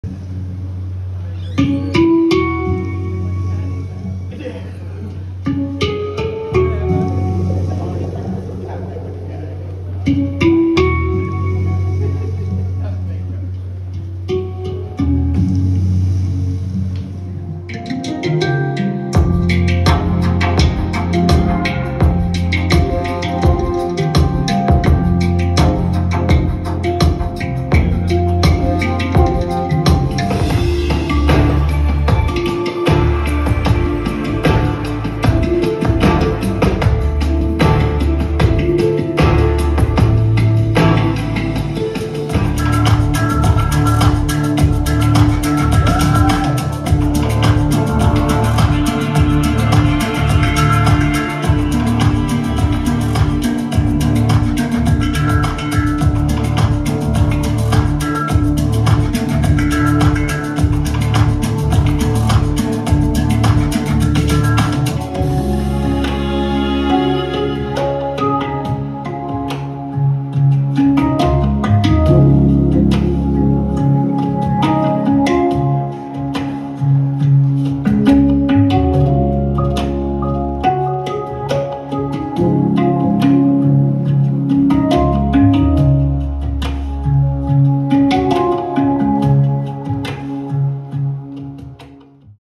Hang Massive - LIVE 2022 (Oxford, UK) - merged short clips
Date: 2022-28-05 Location: "O2 Acadmey 2", City: Oxford, Country: UK
Cam: iPhone 13 mini

Just some nicely merged clips from this lovely gig.